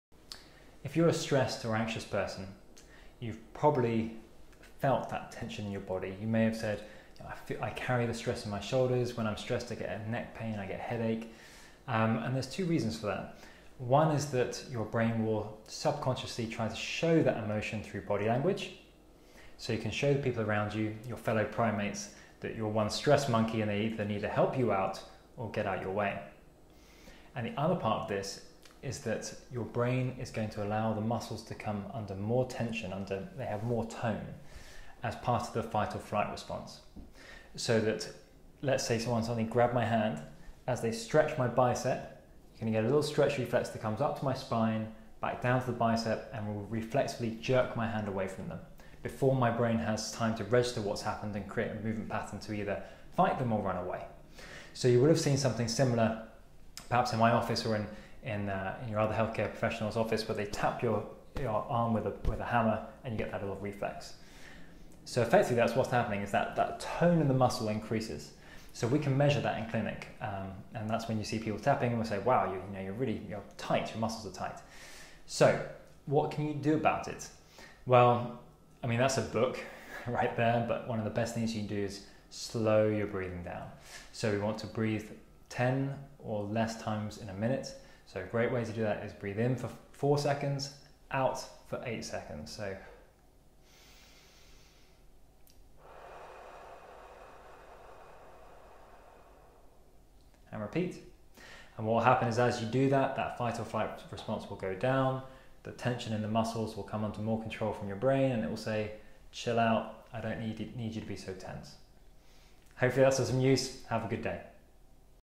Why stress and anxiety cause muscle tension.
Have you ever wondered why being stressed makes you neck, back and shoulders feel so tight? Here’s the reason why!